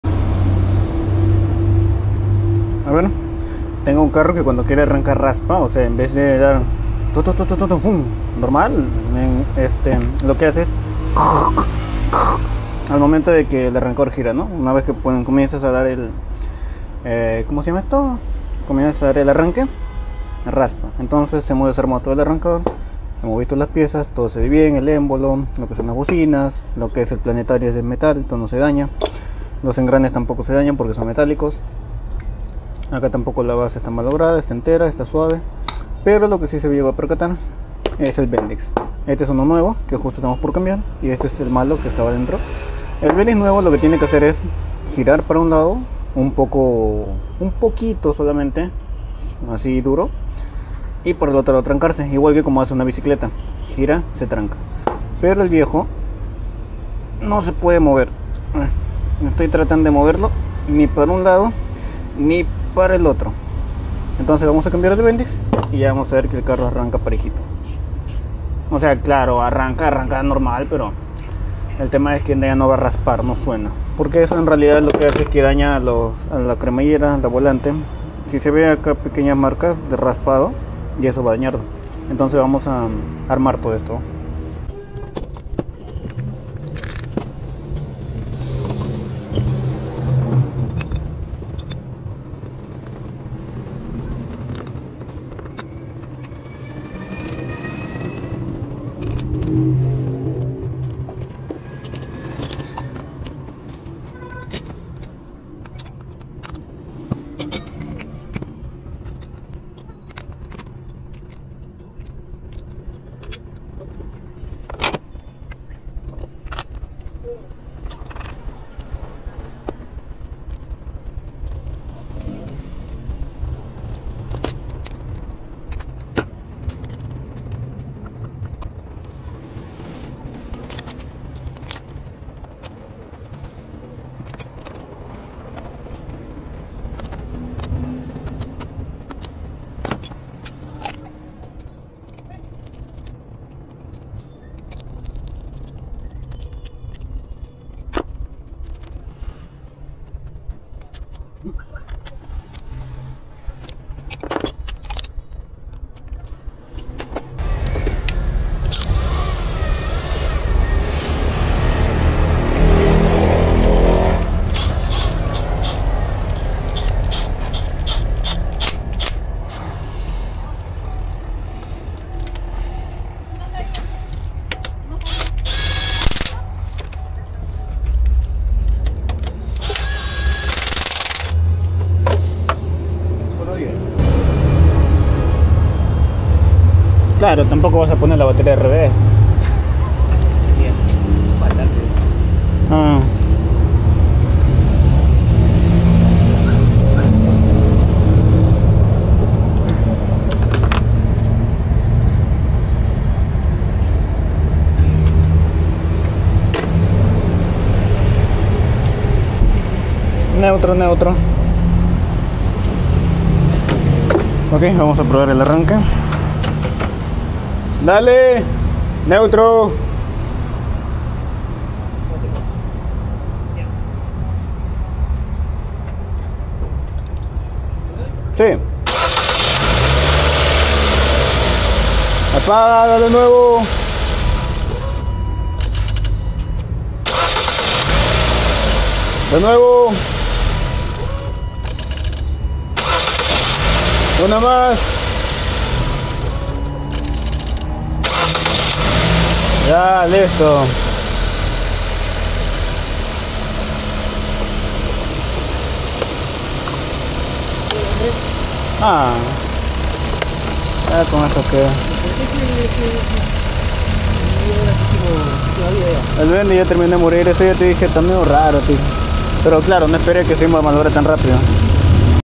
Por qué mi carro raspa al encender?
Olvide grabar el comienzo para poder ejemplificar pero cuando hay un sonido metálico de raspado al momento de arrancar, es bastante notorio, muchas veces se da el caso de que sea el bendix como ahora, también ocurre cuando esta sin hacer mantenimiento y se atora por as decirlo las piezas dentro del arranque

Si te gustaron alguna de las herramientas que utilice en el video, te dejo el link aquí abajo: